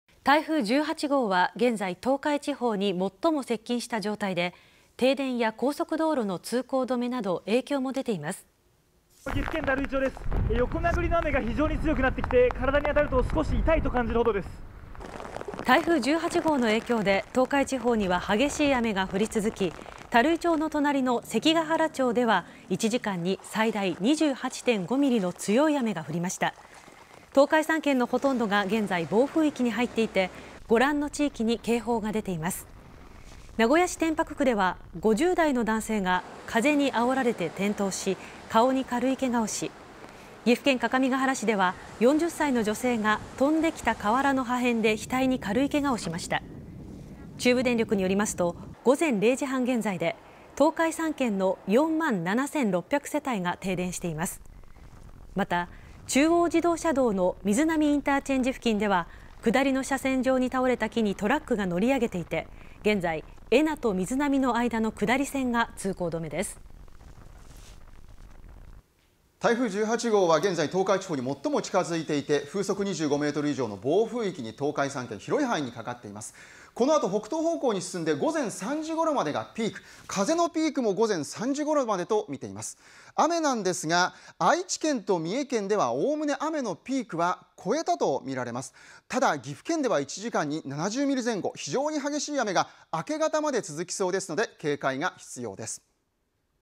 台風１８号‥東海地方は暴風域に、けが人も
台風１８号は、東海地方に最も接近した状態で、停電や高速道路の通行止めなど、影響が出ています。

　　「岐阜県垂井町です。
　横殴りの雨が降っていて、身体に当たると痛いくらいです」
　　　（竹田記者）

　台風１８号の影響で、東海地方には激しい雨が降り続き、垂井町の隣の関ヶ原町では、１時間に最大２８．５ミリの強い雨が降りました。

　東海３県のほとんどが、暴風域に入っていて、広い範囲に警報が出ています。

　名古屋市天白区では、７０代の男性が、風にあおられて転倒し、顔に軽いけがをしました。

　岐阜県各務原市では、４０歳の女性が、飛んできた瓦の破片で額に軽いけがをしました。

　中部電力によりますと、午前１時現在で、東海３県の４万７２９０世帯が停電しています。

　また、中央道の瑞浪インターチェンジ付近では、下りの車線上に倒れた木に、トラックが乗り上げて、恵那と瑞浪の間の下り線が、一時通行止めになりました。

　台風１８号は、午前１時現在、最も東海地方に接近していて、風速２５メートル以上の暴風域に東海地方が入っています。

　台風は、東北に進んでいて、午前３時ごろまでが風のピークとなります。

　三重県と愛知県では、概ね雨のピークは終えたと見られますが、岐阜県では７０ミリ前後の雨が、１８日の明け方まで続く可能性があるため、警戒が必要です。